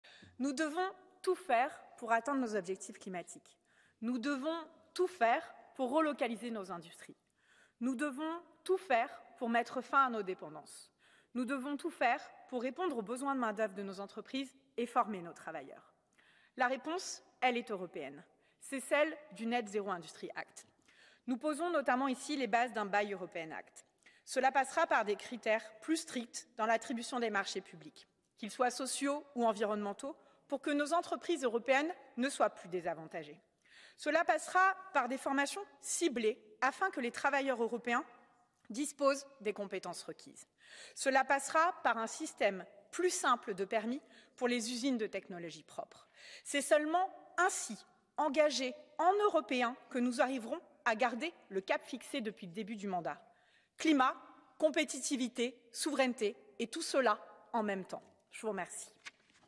Climat, compétitivité, souveraineté : tout cela en même temps | Marie-Pierre Vedrenne
Marie-Pierre Vedrenne, députée européenne MoDem-PDE, est intervenue en séance plénière au Parlement européen sur le Net-Zero Industry Act. Revoir son intervention.

🔗 Pour en savoir plus, consultez l'article complet sur le site du MoDem